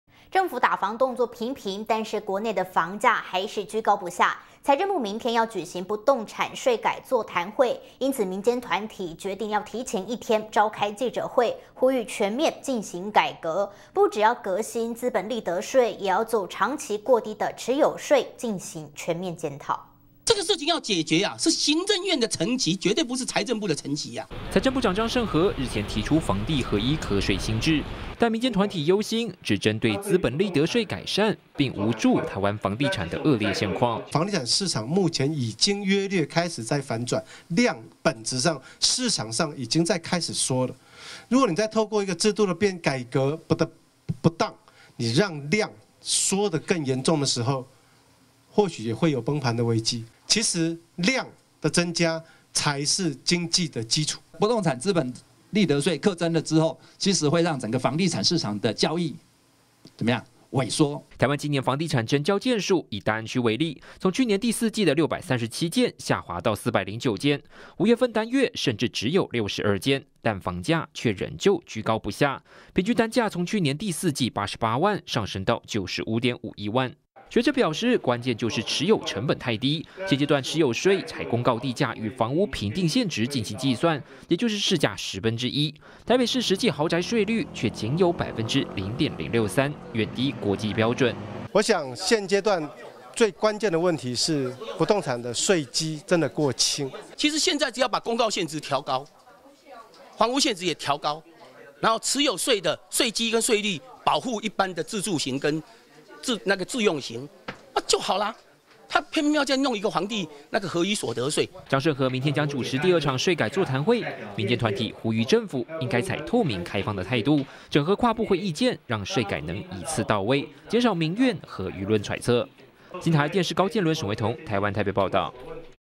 【新唐人/NTD】不動產稅制一團亂 民間呼籲全面改革｜財政部｜不動產稅｜資本利得稅｜稅改
政府打房動作頻頻，但國內房價仍舊居高不下，財政部明天(19日)舉行不動產稅改座談會，民間團體決定提前一天召開記者會，呼籲全面進行改革，不只革新資本利得稅，也要就 長期過低的持有稅 進行全面檢討。